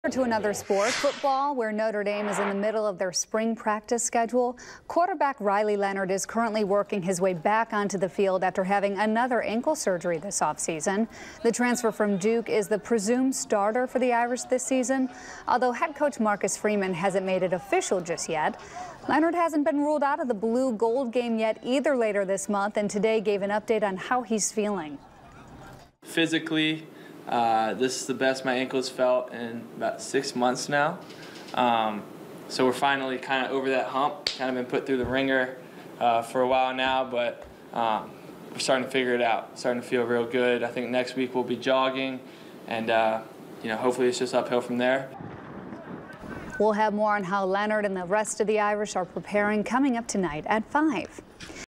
Notre Dame QB Riley Leonard gives update on health after second ankle surgery
Quarterback Riley Leonard is currently working his way back onto the field after having another ankle surgery this offseason.